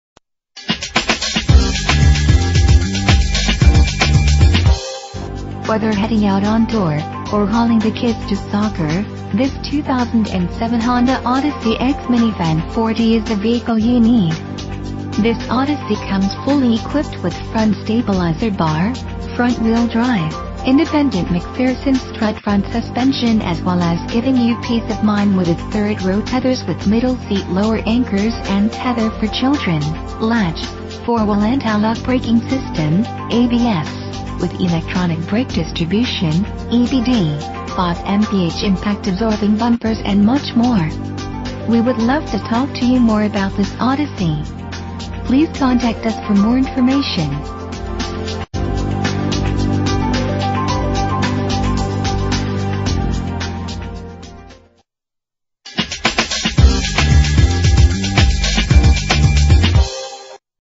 2007 Honda Odyssey - Hamilton NJ
Year: 2007
Make: Honda
Model: Odyssey
Trim: EX Minivan 4D
Engine: 6 cylinder 3.5 liter vtec
Transmission:
Color: Blue
Mileage: 31410
Address:
655 US Highway 130